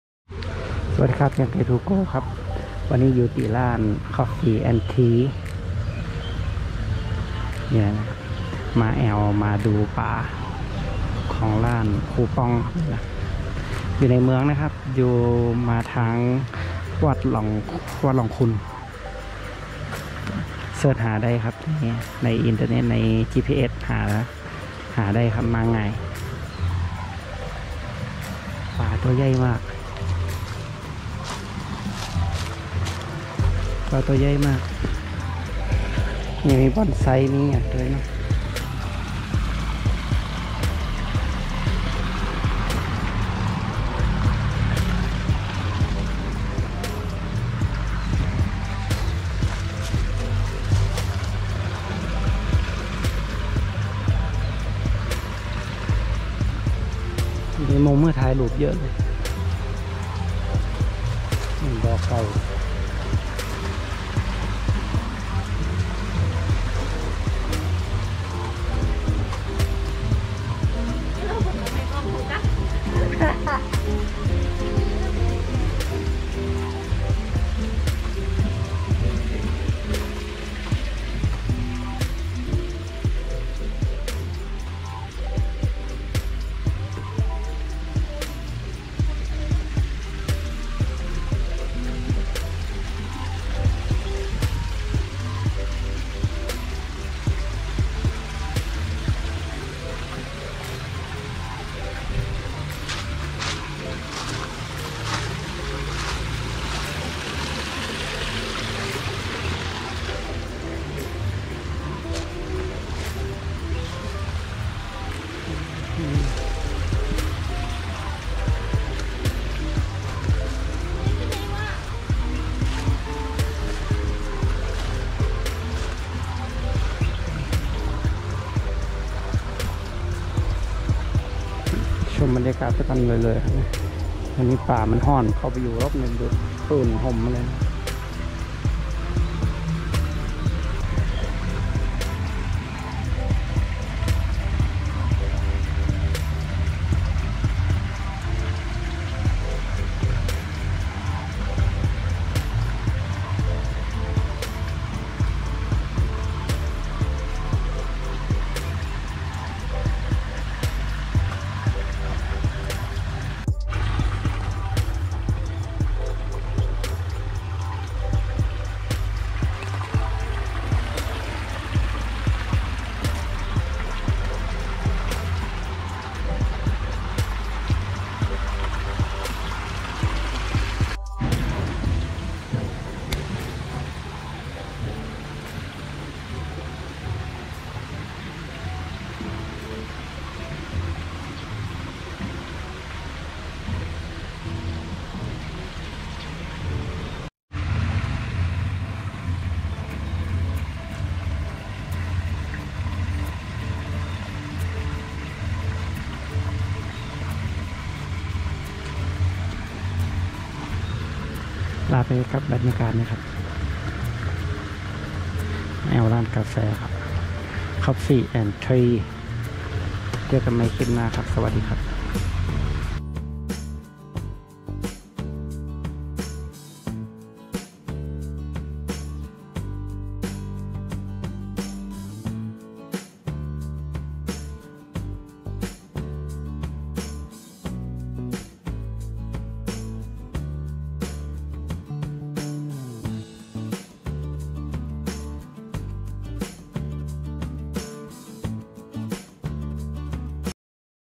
ชิมกาแฟชมปลาสวยๆที่ร้าน#coffeetree coffee&cake# EP.144
ร้านใกล้ๆตัวเมืองไปง่ายๆบรรยากาศดีมีมุมให้ถ่ายรูป กาแฟอร่อยครับ